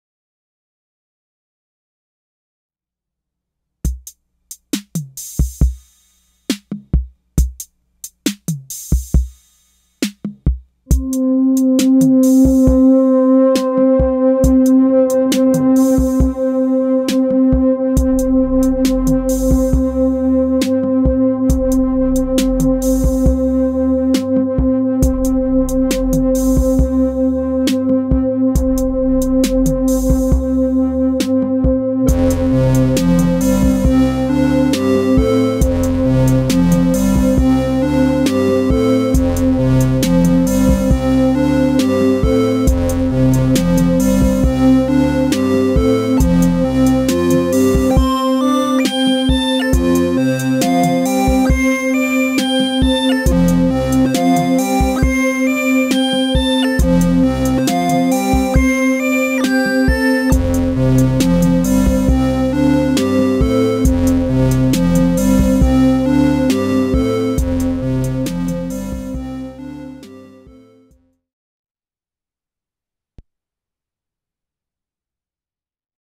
Sessions: Jamuary 2019 (Feat. Roland TR-606, MC-202, and Elektron Digitone)
Hey! This is a Jamuary contribution for 2019. She's simple but she's nice ;). Roland TR-606 and Roland MC-202 Din-Sync'd via the Elektron Digitone MIDI Out and Thru. Thanks for stopping by!